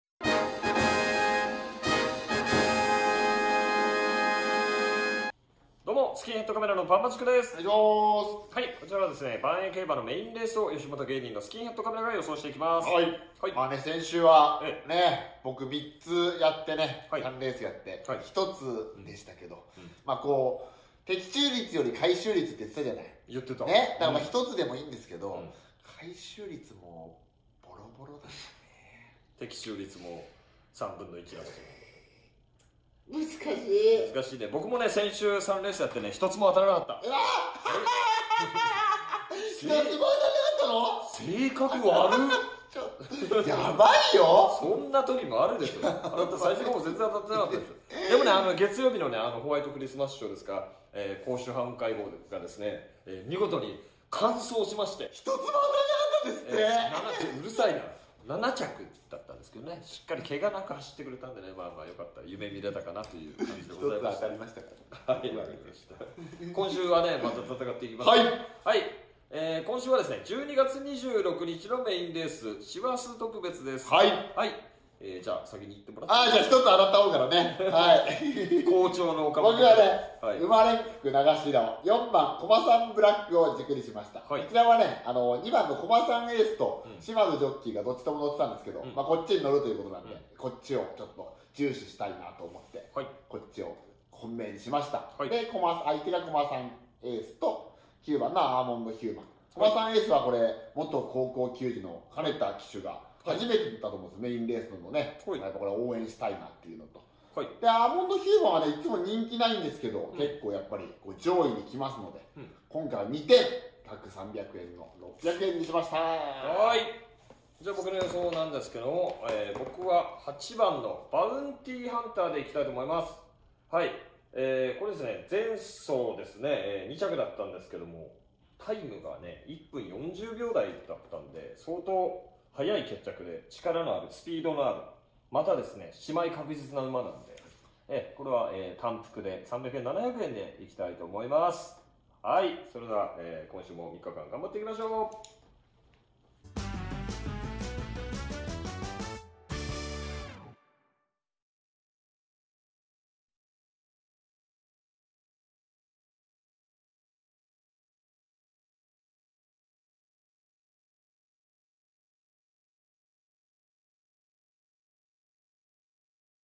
【ばんえい予想】ばんば塾～12月26日 コンビ編～
【ばんえい予想】スキンヘッドカメラの「ばんば塾」
吉本芸人スキンヘッドカメラがばんえい競馬のメインレース予想を毎週更新中！

■出演者
・スキンヘッドカメラ　岡本

・スキンヘッドカメラ　シモ

スキンヘッドカメラも出演する、札幌よしもとのYouTubeチャンネル